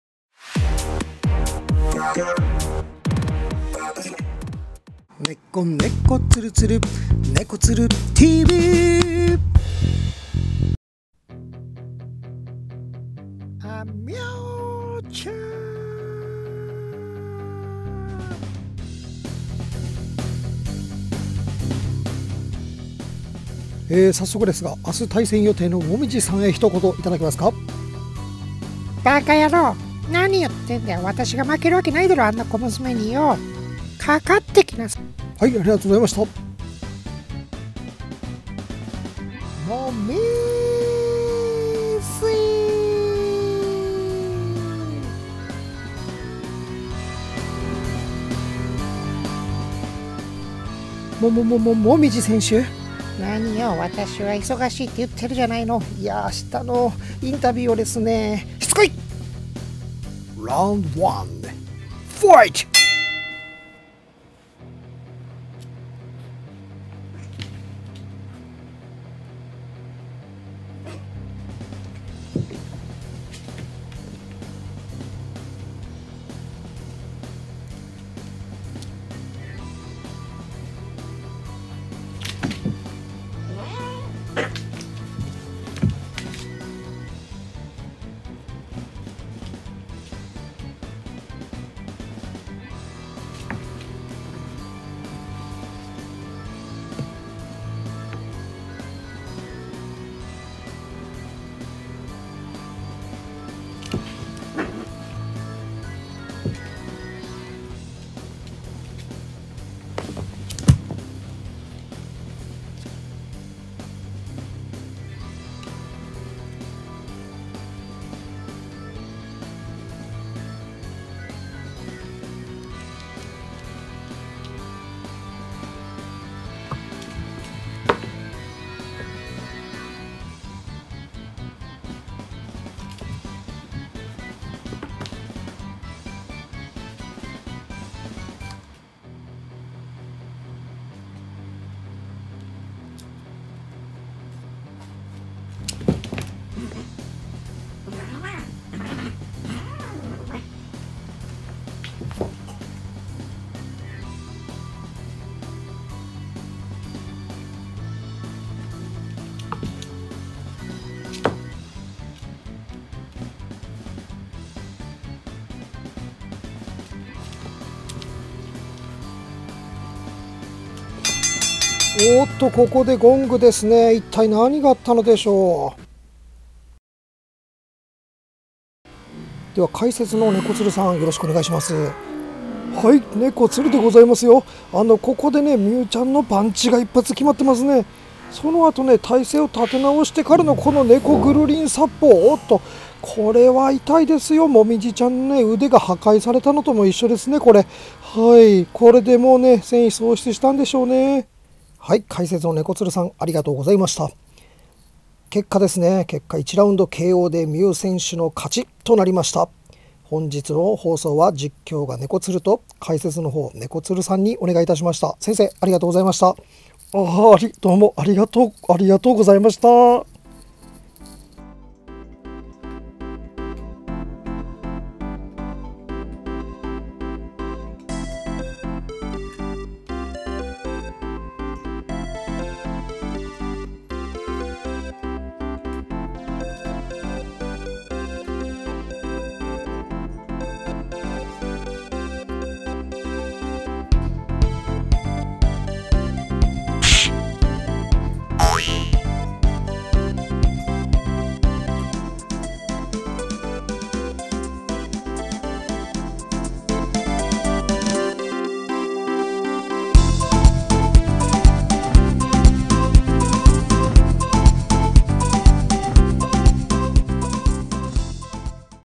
【猫 喧嘩】母と娘が親子でケンカを始めた〜プロレス風味〜【ねこつる】
うちのかわいい女子猫を見て！
親子喧嘩さえも可愛い！
シルバーベンガルの母と娘がケンカしていたのでプロレス風味に仕上げてみました。
あくまでも風味です。
自然な猫の姿を捉えた動画制作も好きですが、僕はやっぱり明るく楽しい動画作りが好きなんだな、と思いました。
まだまだ動画初心者ですが明るく楽しい動画でうちのかわいい猫を紹介させてください！

ねこつるTVでは、面白く楽しく猫の生態がちょっとだけ学べるチャンネル作りに励んでます！
あなたの登録お待ちしてます❗️

猫 おもしろ